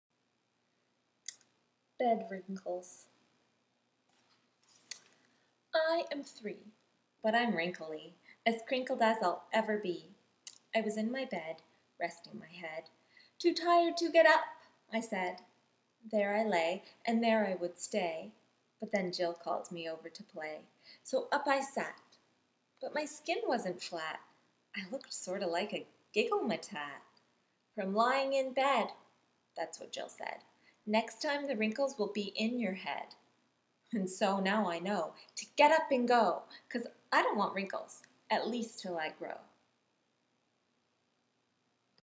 Children's Poem: Bed Wrinkles
A children's poem about the wrinkles you get on your face when you lie in bed.